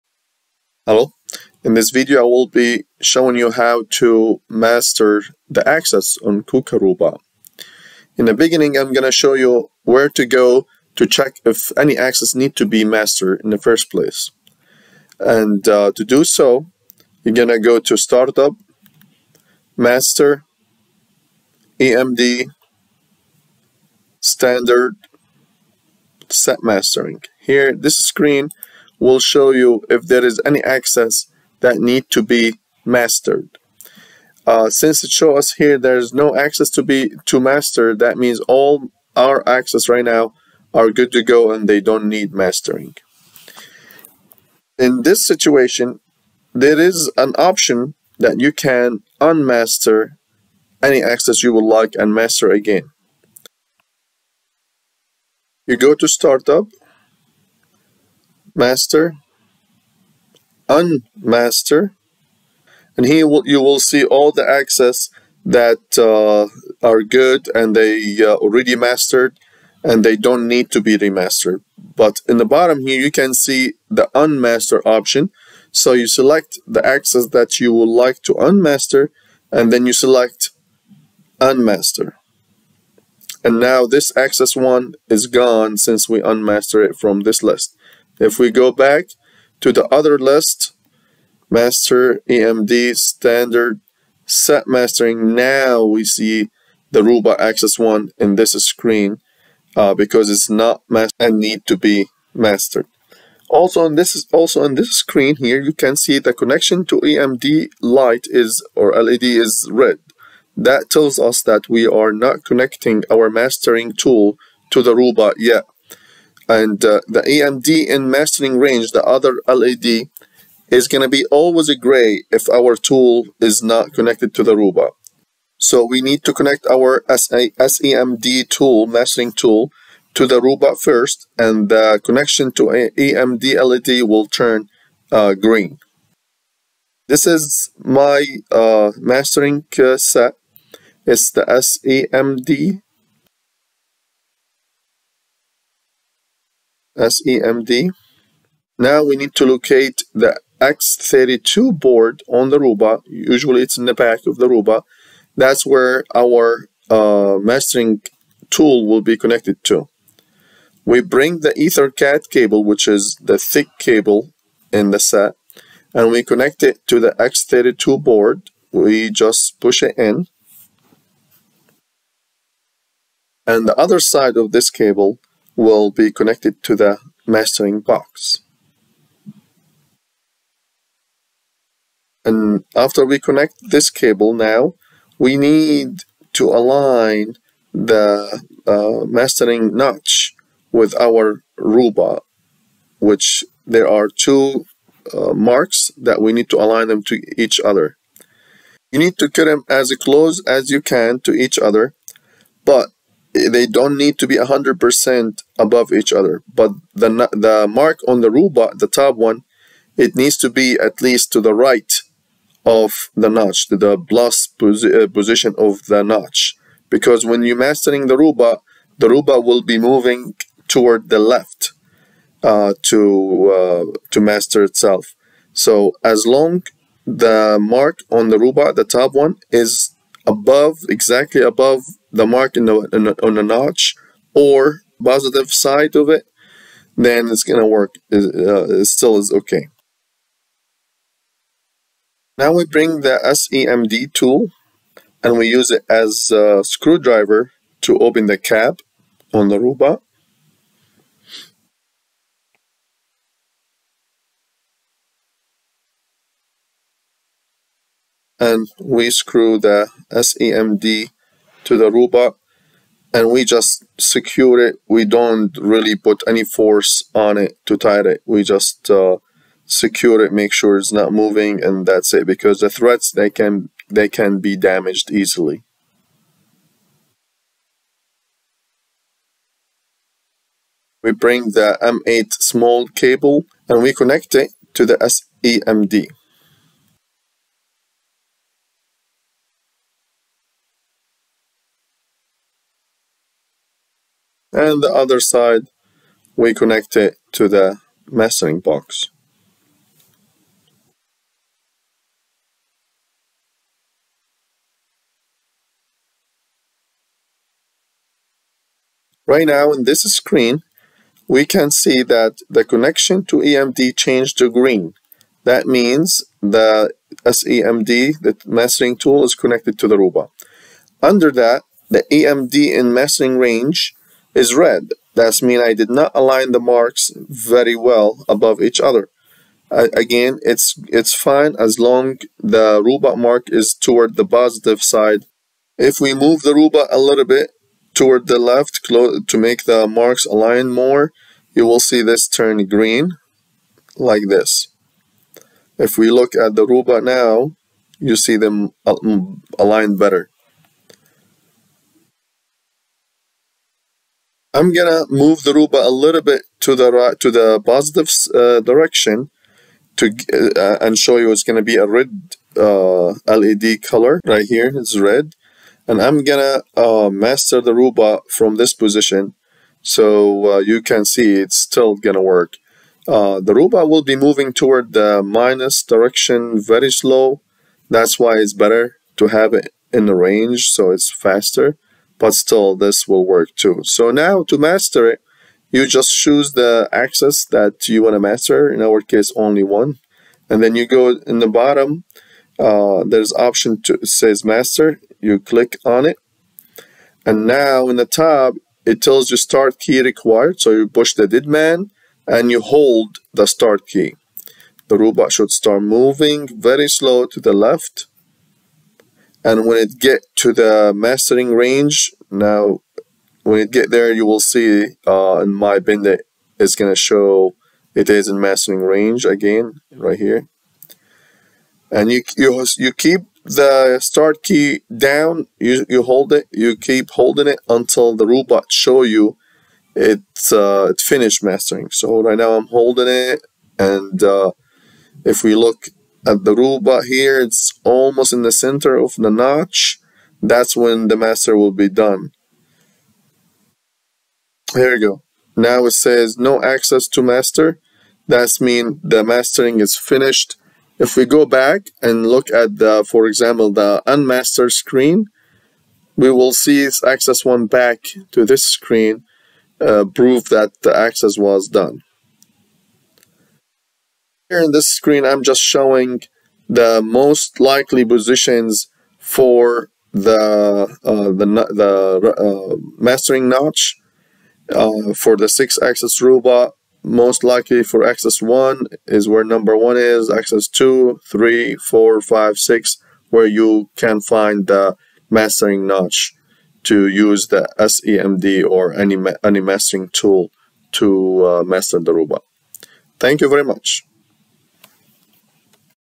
Kuka Axes Mastering Using SEMD Mastering Tool
Here you can learn how to master or unmaster any axis on KUKA robot using the SEMD tool.